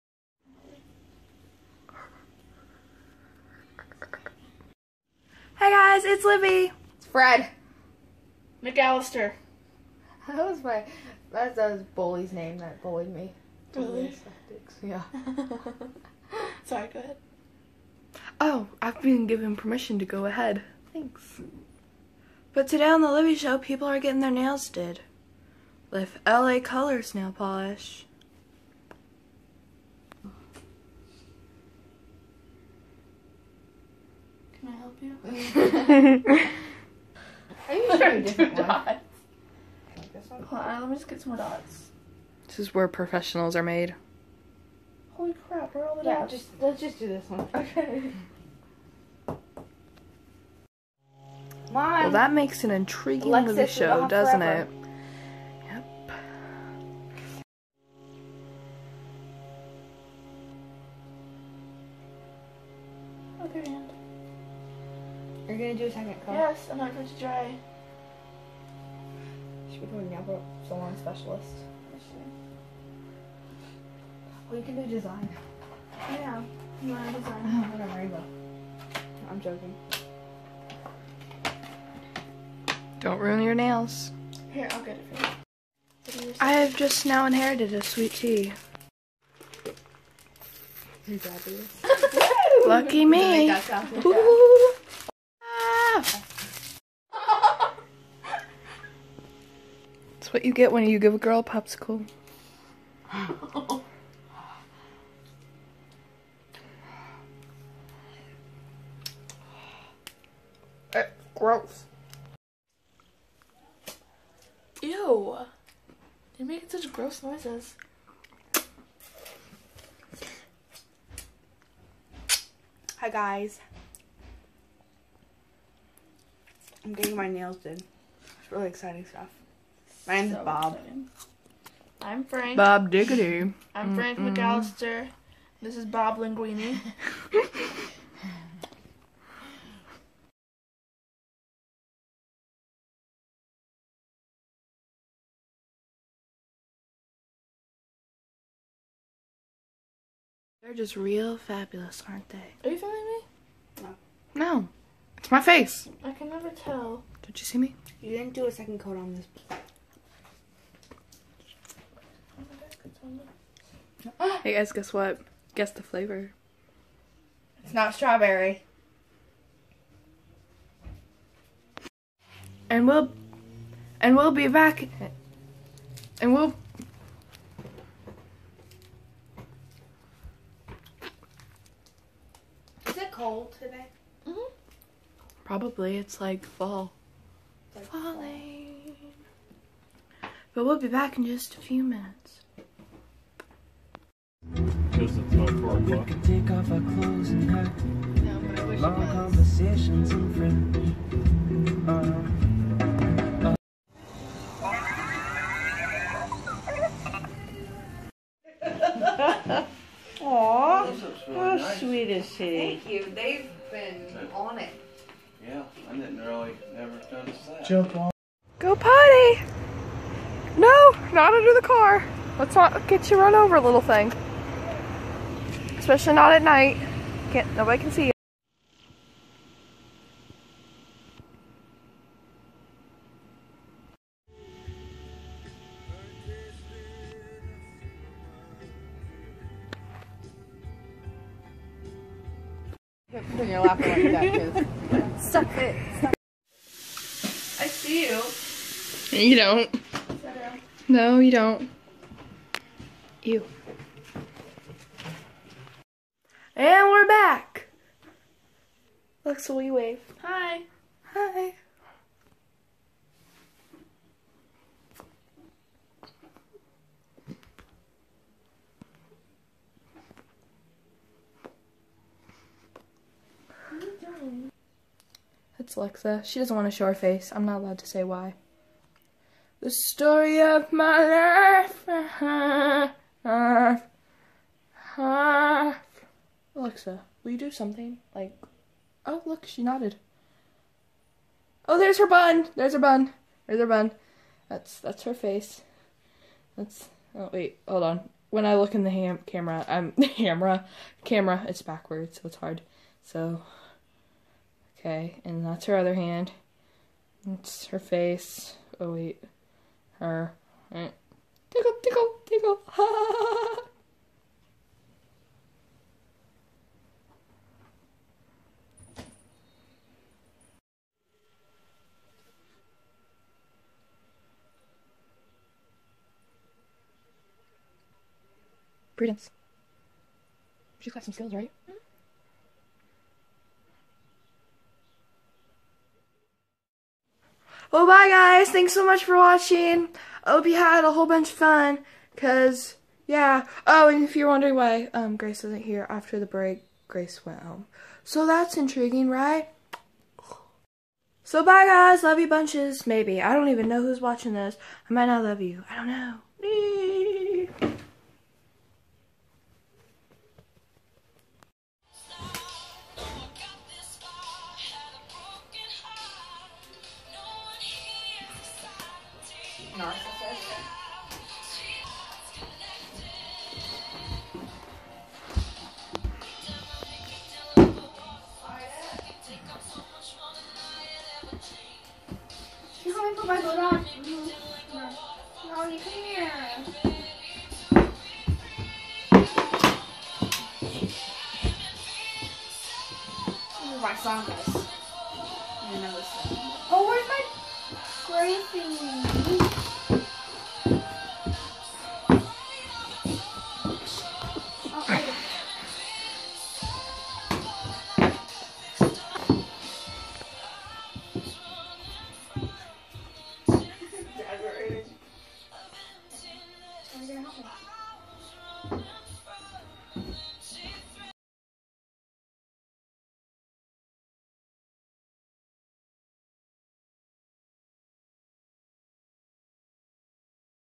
Livvie Show 11 (Getting Our Nails Did)
For the,
First half: Grace gets her nails did and we eat popsicles.
Second half: Alexa gets embarrassed and hides her face.